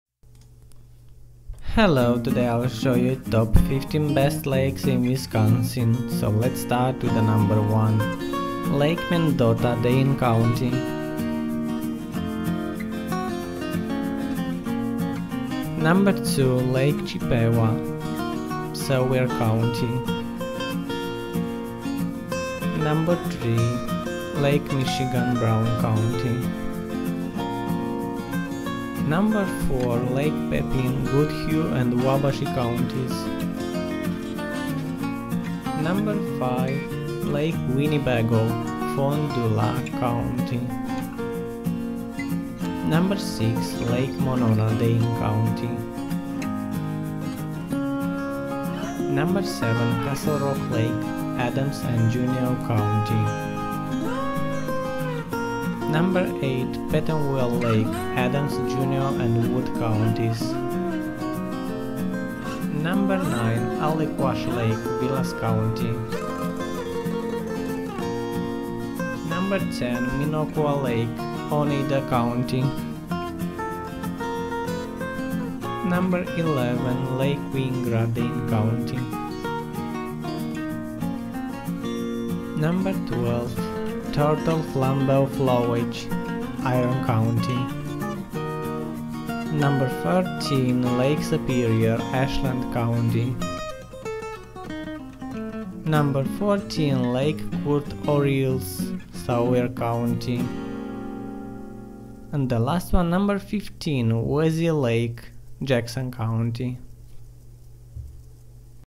Top 15 Best Lakes In Wisconsin
Here are top 15 best lakes in Wisconsin
1. Lake Mendota, Dane County - Reel Wave Media / shutterstock
2. Lake Chippewa, Sawyer County - Hoogz Photography / shutterstock
3. Lake Michigan, Brown County - Kenneth Keifer / shutterstock
4. Lake Pepin, Goodhue and Wabasha counties
5. Lake Winnebago, Fond du Lac County - Henryk Sadura / shutterstock
6. Lake Monona, Dane County - Suzanne Tucker / shutterstock
7. Castle Rock Lake, Adams and Juneau County - Michael Rolands / shutterstock
8. Petenwell Lake, Adams, Juneau, and Wood counties - Earl D. Walker / shutterstock
9. Allequash Lake, Vilas County
10. Minocqua Lake, Oneida County - Jason Patrick Ross / shutterstock
11. Lake Wingra, Dane County - youngryand / shutterstock
12. Turtle-Flambeau Flowage, Iron County
13. Lake Superior, Ashland County - nikitsin.smugmug.com / shutterstock
14. Lac Courte Oreilles, Sawyer County
15. Wazee Lake, Jackson County - Joe Ferrer / shutterstock